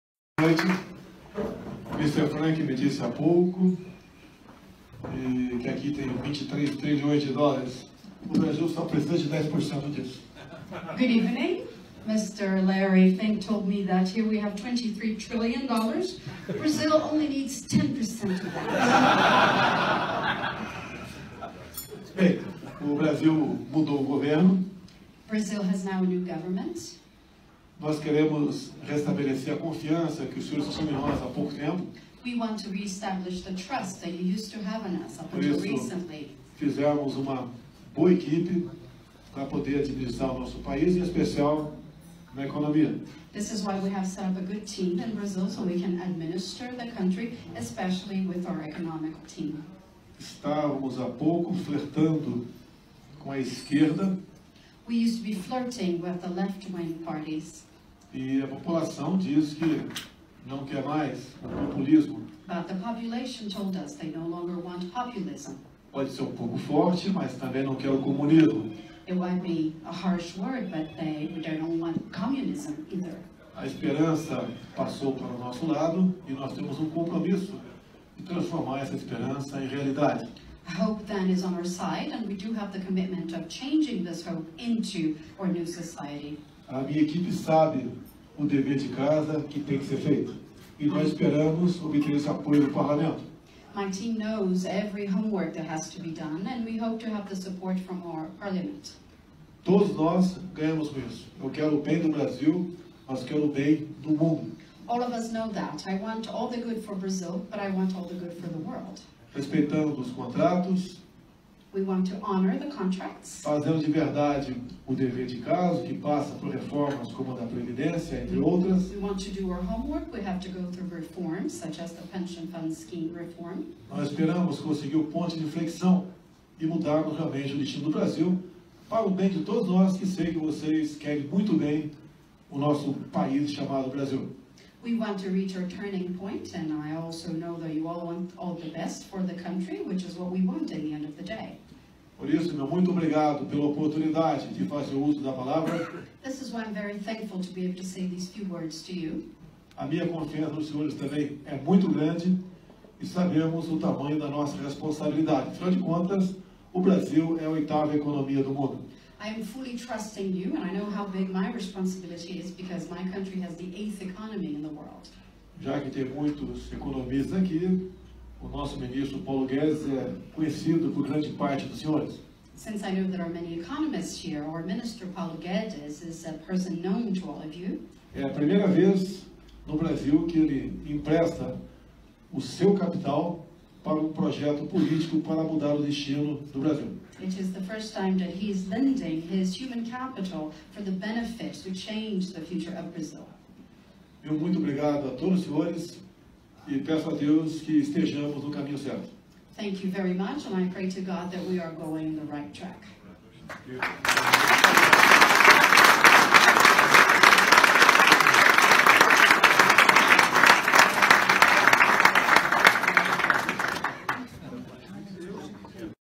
Com Muita Humildade Bolsonaro discursa para dezenas de Ministros da Economia
Com Muita Humildade Bolsonaro discursa para dezenas de Ministros da Economia e fundos trilionários em Davos!